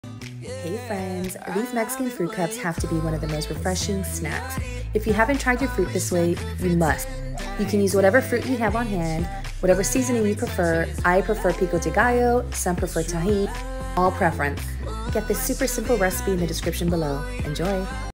Mexican Fruit Cups | have you tried your fruit this way?
Mexican Fruit Cups

This classic side dish is a staple in my household. I remember visiting and walking the streets of Mexico grabbing a cup or two. In the United States, you typically find these clear plastic fruit cups at street corners, in grocery stores and many other areas where Mexican markets and restaurants reside. They are generally cubed or cut into spears.

✨Simple Recipe ⬇️
cucumbers
mango
tangerines/oranges
watermelon
pineapple
lime, freshly squeezed
pico de gallo seasoning mix

🍉 These refreshing cups can be made with whatever fruit you have on hand. They vary by season and vendor. I like to mix it up with different fruits and veggies. Add freshly squeezed lime and pico de gallo.
🌶 Pico de Gallo powder is the preferred chile (chili) we use. Some prefer Tajin, Chamoy and other Mexican seasonings. All preference. Just sprinkle it on top of the fruit, add salt to taste and mix it all up to combine. Enjoy!

Many blessings friends,
Rese 💛